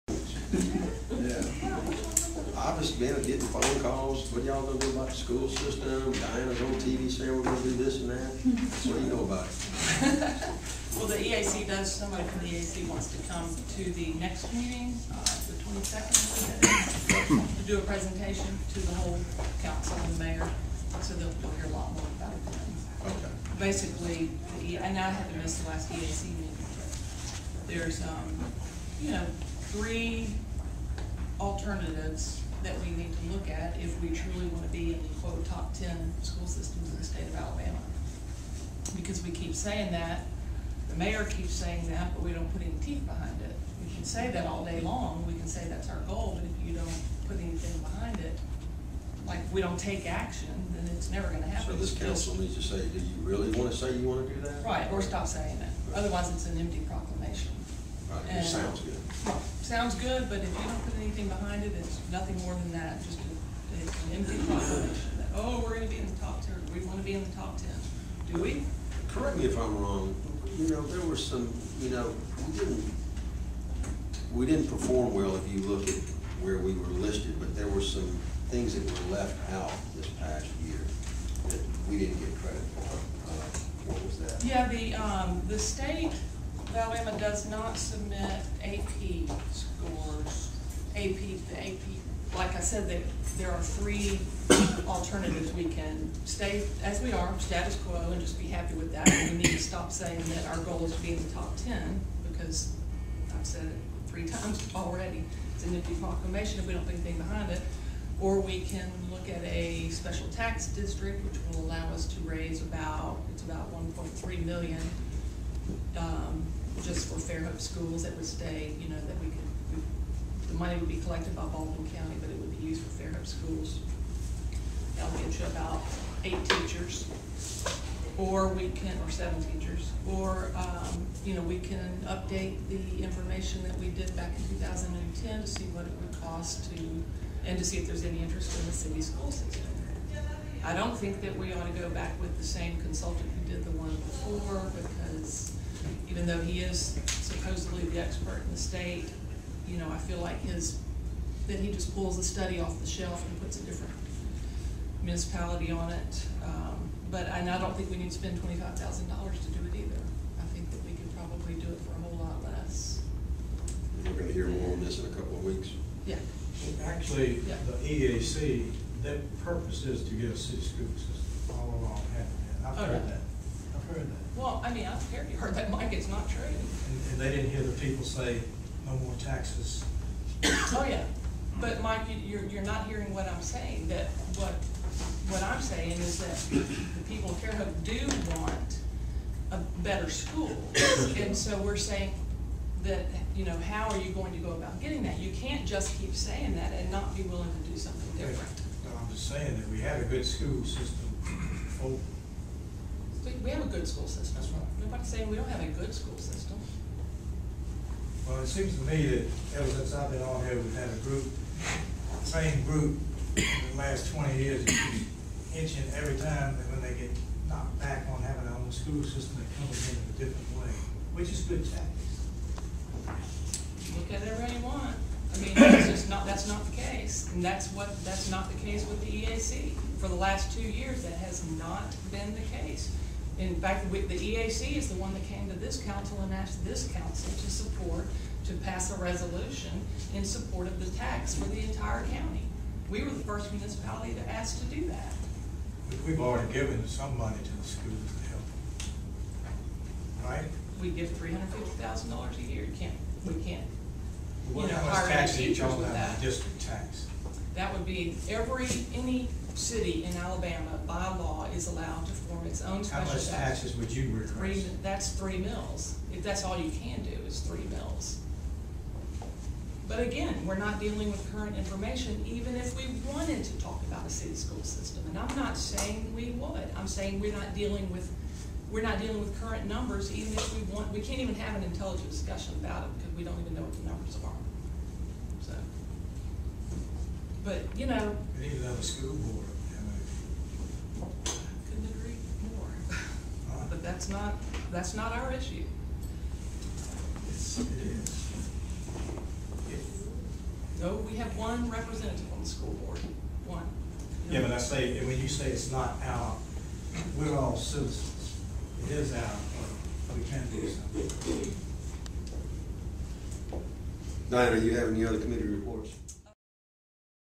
Council members debate school options. (thefairhopetimes.blogspot.com)
At a work-session Monday, council members talked about options for providing supplementary funding to county schools in the city-area..